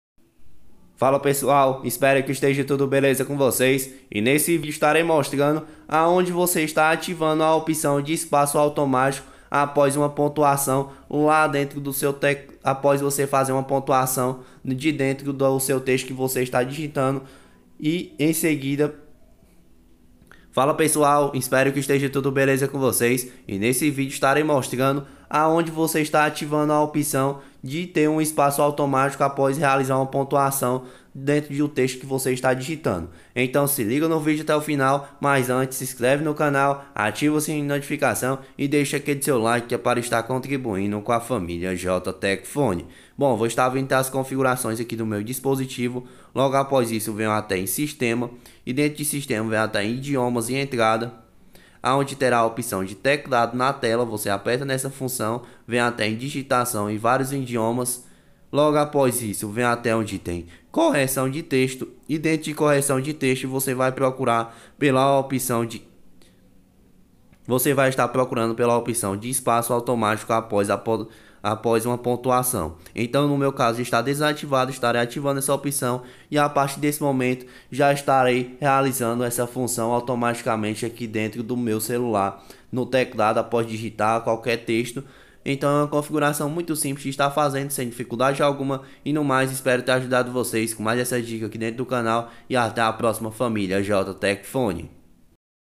COMO ATIVAR A OPÇÃO DE ESPAÇO AUTOMÁTICO APÓS UMA PONTUAÇÃO (Configuração do Motorola)
eai família.
nesse vídeo você vai saber como está ativando a opção de pontuação logo após um espaço no teclado do celular motorola.